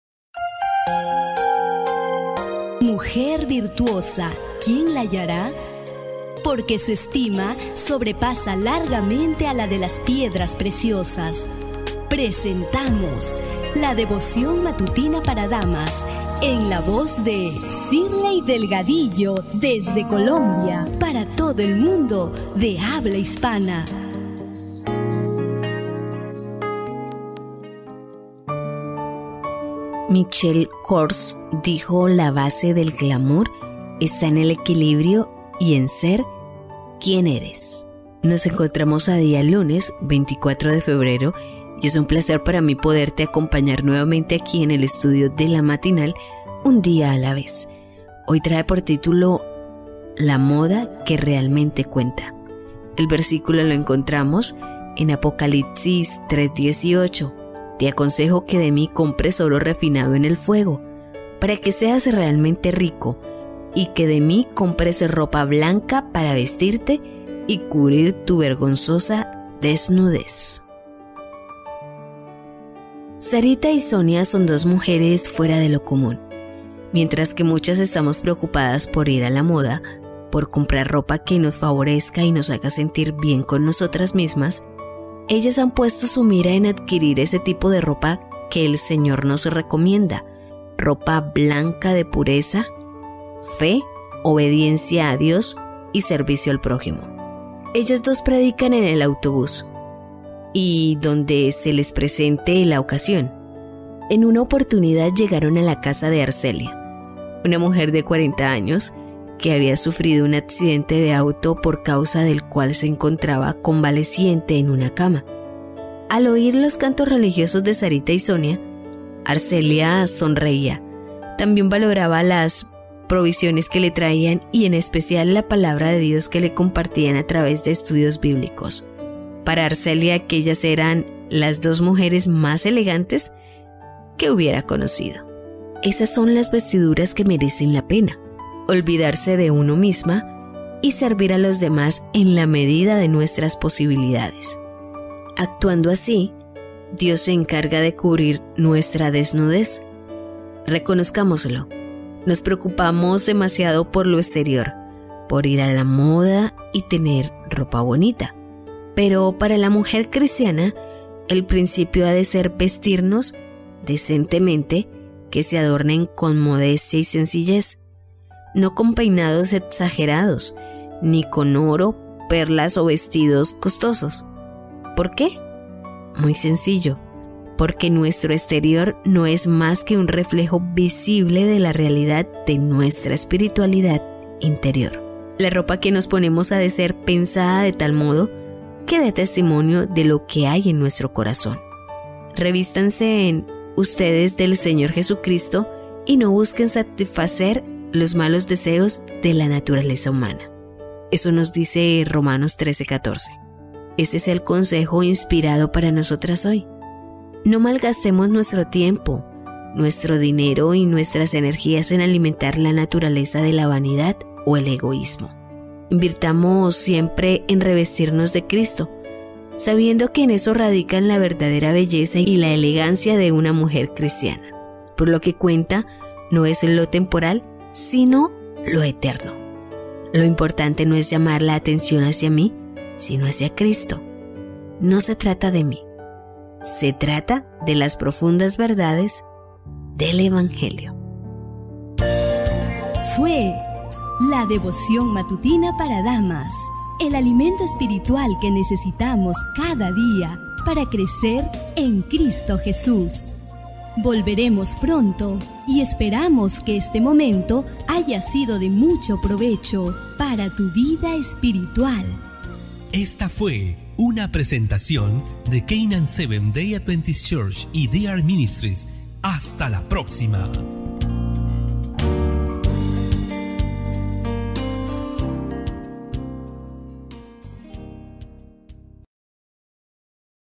Devoción Matutina Para Damas - 24 de Febrero del 2020
LA MODA QUE REALMENTE CUENTA

«Te aconsejo que de mi compres oro refinado en el fuego, para que seas realmente rico; y que de mi compres ropa blanca para vestirte y cubrir tu vergonzosa desnudez»

(Apoc. 3:18).

Sarita y Sonia son dos mujeres fuera de lo común. Mientras que muchas estamos preocupadas por ir a la moda, por comprar ropa que nos favorezca y nos haga sentir bien con nosotras mismas, ellas han puesto su mira en adquirir ese tipo de ropa que el Señor nos recomienda: ropa blanca de pureza, fe, obediencia a Dios y servicio al prójimo.

Ellas dos predican en el autobús (y donde se les presente la ocasión). En una oportunidad llegaron a la casa de Arcelia, una mujer de cuarenta años que había sufrido un accidente de auto por causa del cual se encontraba convaleciente en una cama. Al oír los cantos religiosos de Sarita y Sonia, Arcelia sonreía; también valoraba las provisiones que le traían y, en especial, la Palabra de Dios que le compartían a través de estudios bíblicos. Para Arcelia, aquellas eran las dos mujeres más elegantes que hubiera conocido.

Esas son las vestiduras que merecen la pena: olvidarse de una misma y servir a los demás en la medida de nuestras posibilidades. Actuando así, Dios se encarga de cubrir nuestra desnudez. Reconozcámoslo: nos preocupamos demasiado por lo exterior, por ir a la moda y tener ropa bonita. Pero para la mujer cristiana, el principio ha de ser vestirnos «decentemente, que se adornen con modestia y sencillez, no con peinados exagerados, ni con oro, perlas o vestidos costosos» (1 Tim. 2:9). ¿Por qué? Muy sencillo: porque nuestro exterior no es más que un reflejo visible de la realidad de nuestra espiritualidad interior. La ropa que nos ponemos ha de ser pensada de tal modo que de testimonio de lo que hay en nuestro corazón.

«Revístanse ustedes del Señor Jesucristo, y no busquen satisfacer los malos deseos de la naturaleza humana» (Rom. 13:14). Ese es el consejo inspirado para nosotras hoy. No malgastemos nuestro tiempo, nuestro dinero y nuestras energías en alimentar la naturaleza de la vanidad o el egoísmo. Invirtamos siempre en revestirnos de Cristo, sabiendo que en eso radican la verdadera belleza y la elegancia de la mujer cristiana. Porque lo que cuenta no es lo temporal, sino lo eterno.

Lo importante no es llamar la atención hacia mí, sino hacia Cristo. No se trata de mí, se trata de las profundas verdades del evangelio.

Un día a la vez
Por: Patricia Muñoz Bertozzi